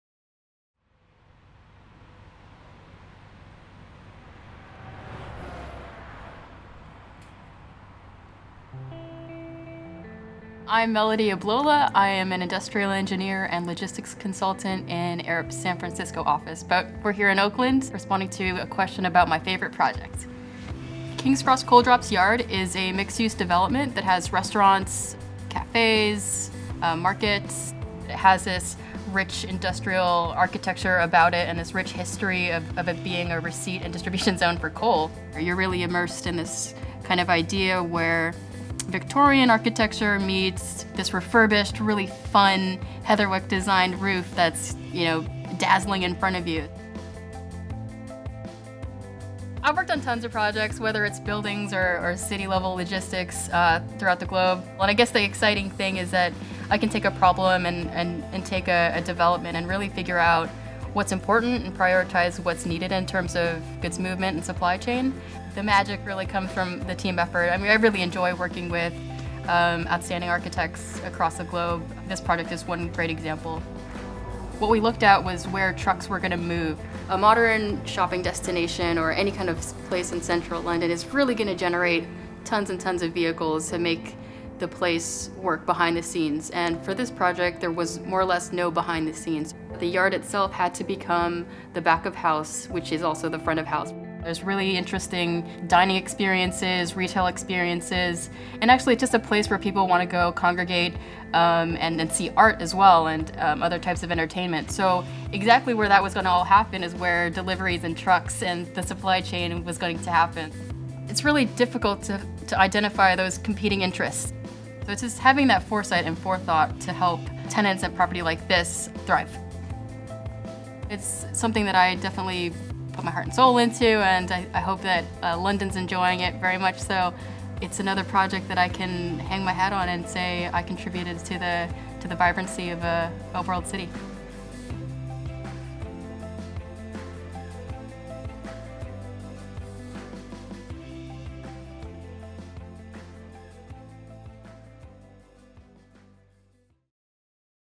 40 Under 40 2019 – Melody Ablola
Melody Ablola is one of 40 architecture, engineering, and construction professionals selected by Building Design+Construction as a 2019 40 under 40 honoree.

MELODY ABLOLA MSC, CMILT
SENIOR LOGISTICS CONSULTANT
ARUP | SAN FRANCISCO
AGE: 36

EDUCATION
MSc, International Transport and Logistics, University of Glamorgan
BS/BA, Industrial & Systems Engineering, University of San Diego

ACCOMPLISHMENTS
Helped develop the waste and deliveries management strategy for the 3,000-unit London 2012 Olympic Athletes Village.
Helped deliver first-of-its-kind automation design for the Zappos.com distribution center, Shepherdsville, Ky.
Helped create fully autonomous solutions for Masdar City (Abu Dhabi), including an underground tunnel network for distribution.

EXTRACURRICULAR
Named role model by Connect Out, Arup’s LGBTQ resource group.
Senior Member, Institute of Industrial Engineers.
Chartered Member, Institute of Logistics and Transport.

OFF THE CLOCK
Hosts Bring Your Own Vinyl community events in Oakland.
Avid gold panner.

SECRET LIFE
Can balance a pile of rocks into a towering sculpture.
Played guitar (Fender Sunburst Telecaster) in an all-girl ska band in high school.